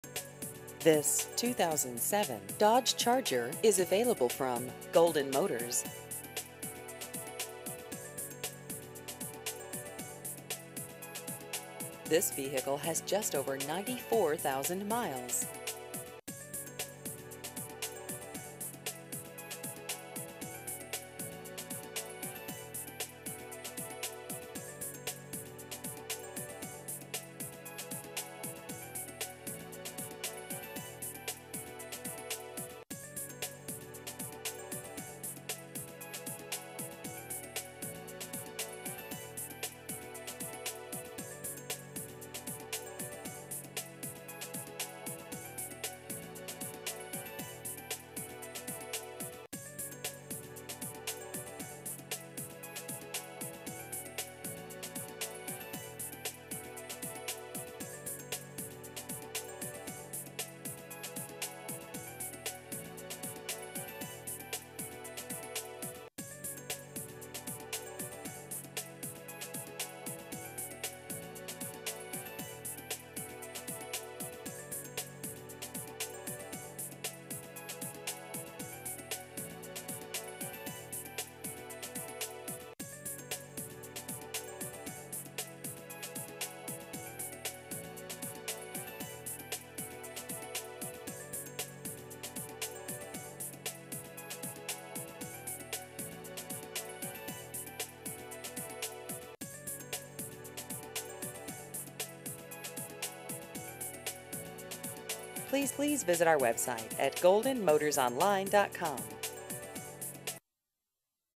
2007 Dodge Charger Used Cars Columbia SC
This 2007 Dodge Charger is available from Golden Motors.